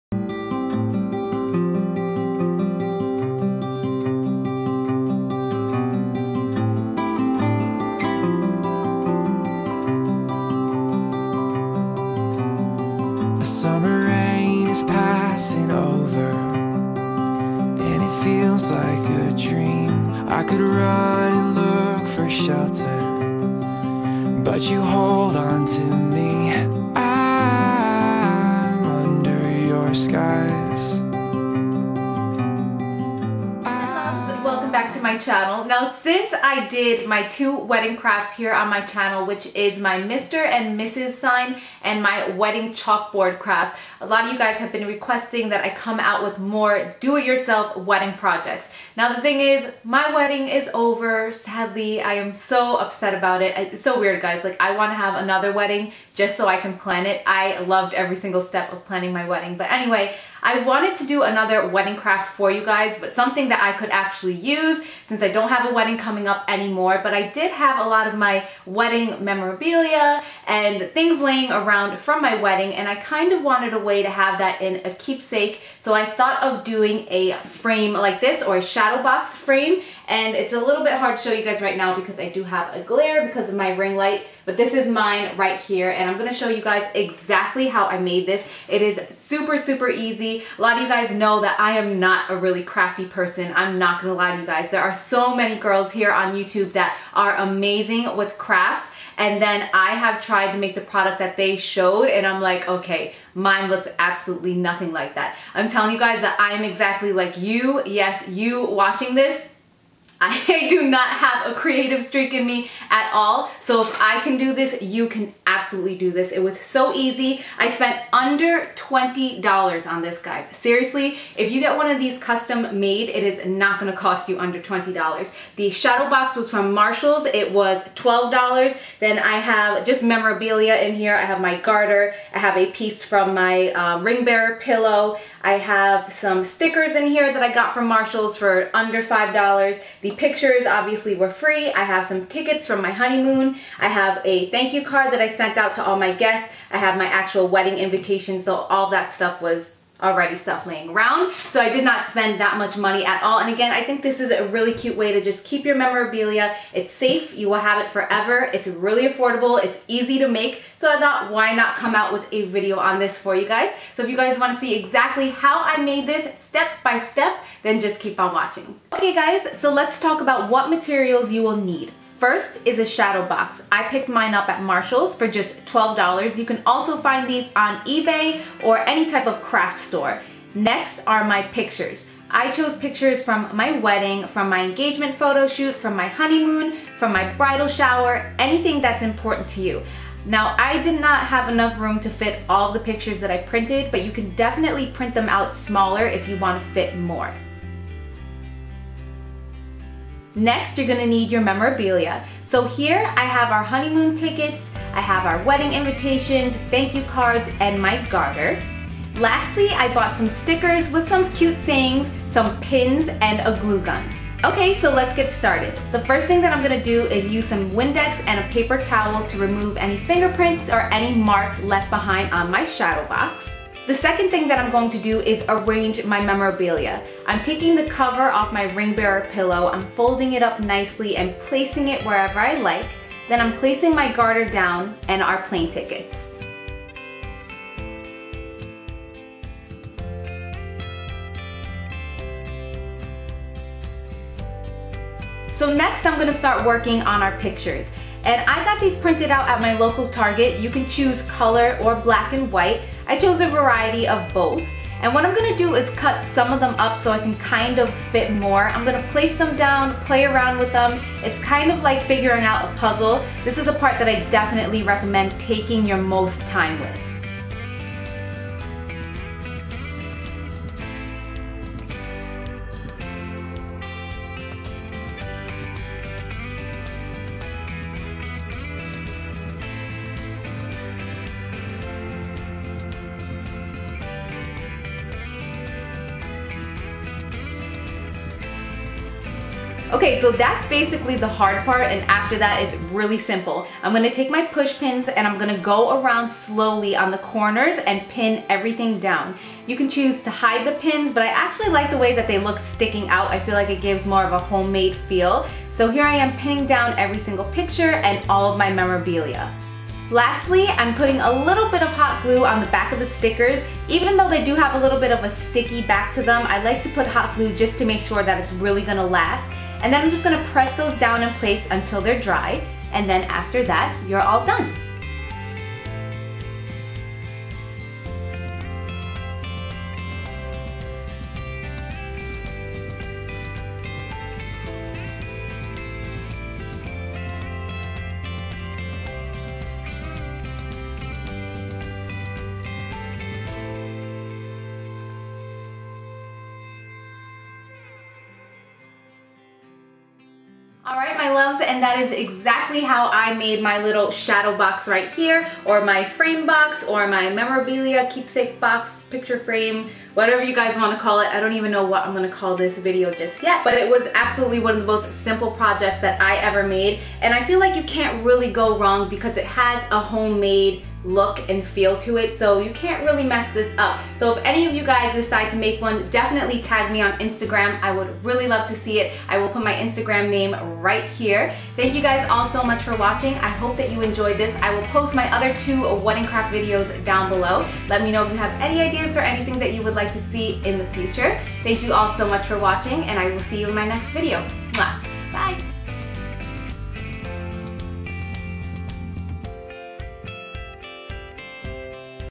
DIY Wedding Project ♥ Shadow Box Frame ♥
Here's a SIMPLE, easy & affordable craft to display & keep your wedding memorabilia safe for many years to come!! :)
You can create a shadow box frame to remember any event!! Anniversaries, vacations, birthdays...the possibilities are endless!! :)

All materials were purchased at Marshalls & Michael's. You can find all you need at any craft supply store or online!!

♡♡♡ MORE INFO ♡♡♡
Don't Forget to click the "LIKE" button so I know you enjoyed this video! I love you guys :)

♥♥♥♥ LINKS ♥♥♥♥